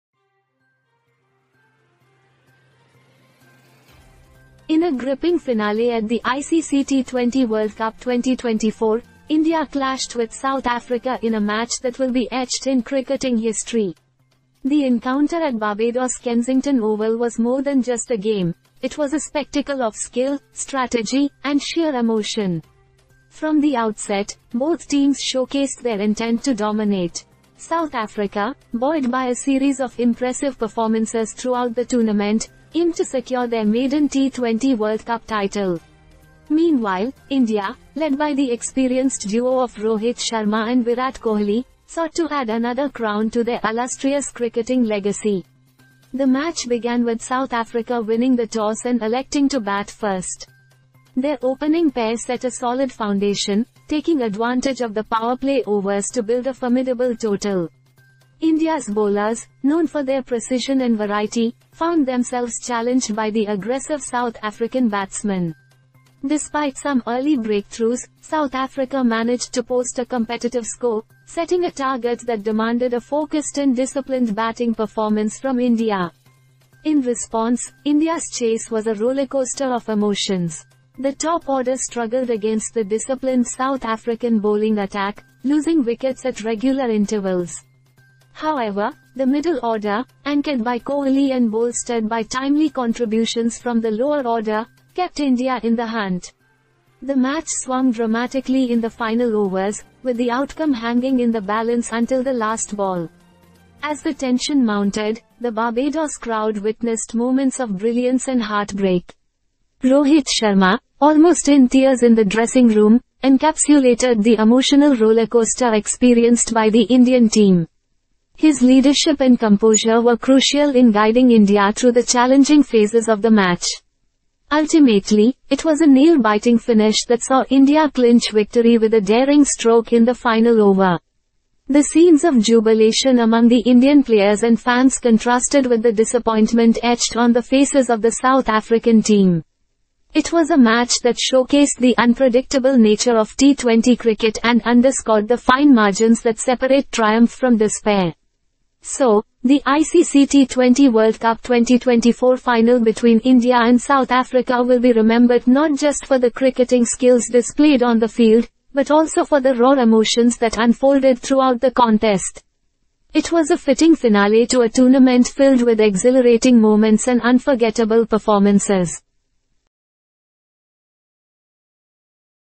T20 World Cup Final: India vs South Africa - Drama and Emotion on the Cricket Field
00:10 T20 World Cup Final: India vs South Africa - Drama and Emotion on the Cricket Field